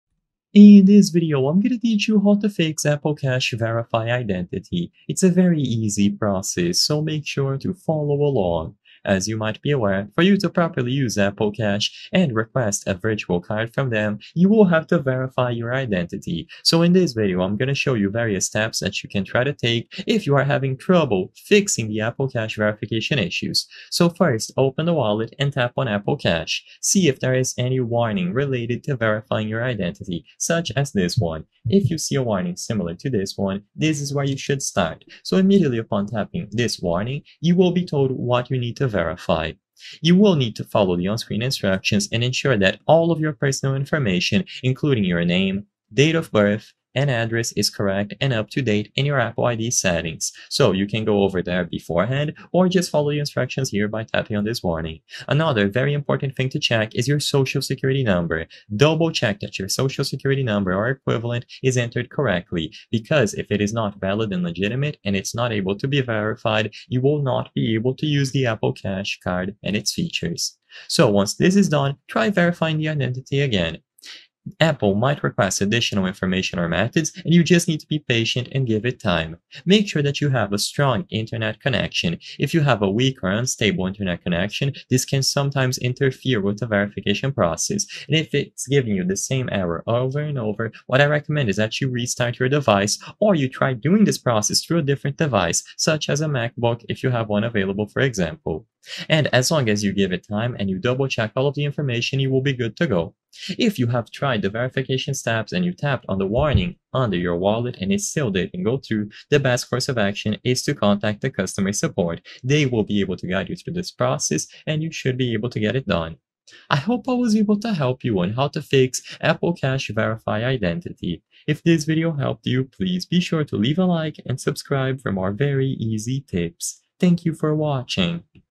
How To FIX Apple Cash Verify Identity (2025) - Step by Step
In this video I will clear your doubts about how to fix apple cash verify identity, and whether or not it is possible to do this.